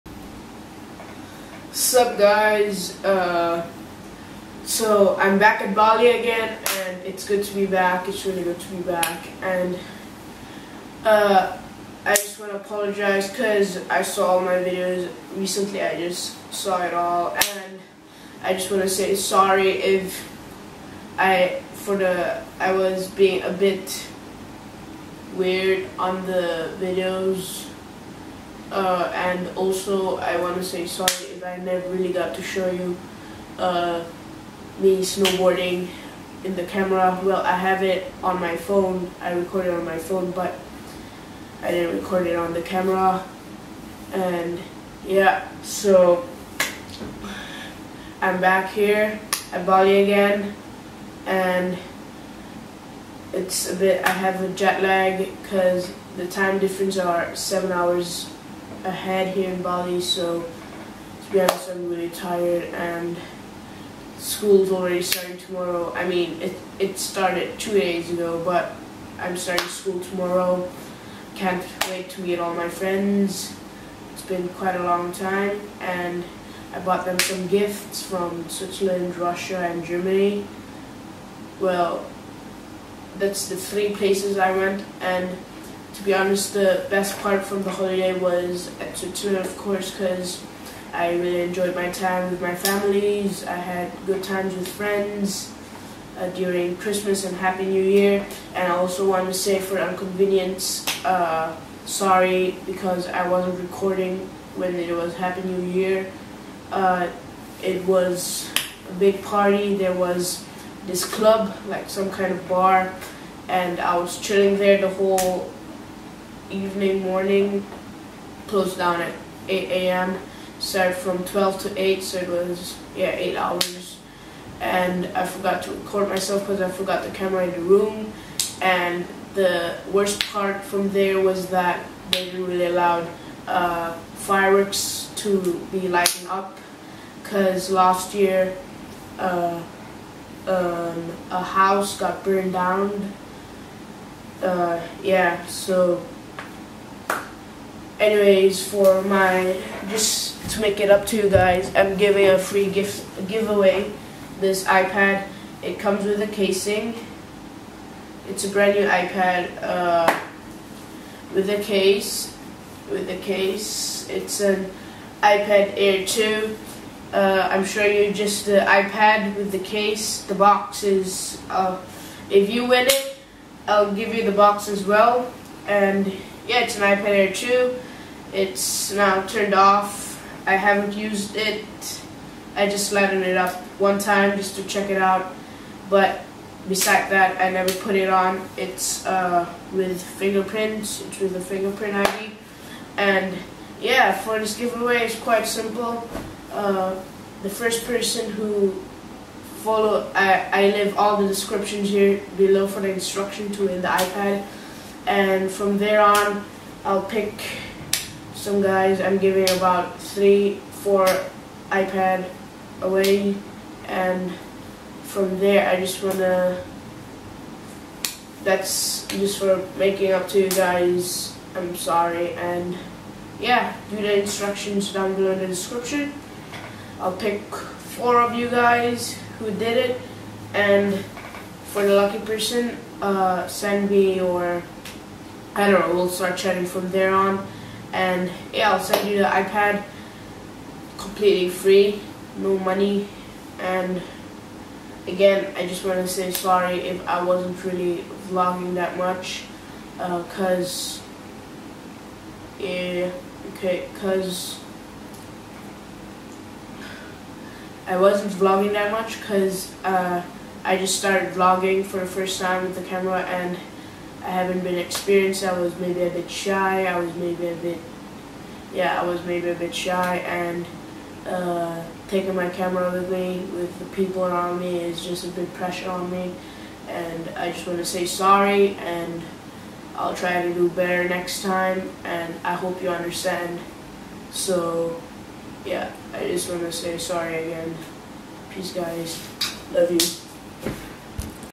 GIVEAWAY...!!!!
i give this giveaway to say sorry for not vlogging properly! it was my first vlog and i was alittle bit on preassure. so yeah this giveaway goes to u guys:)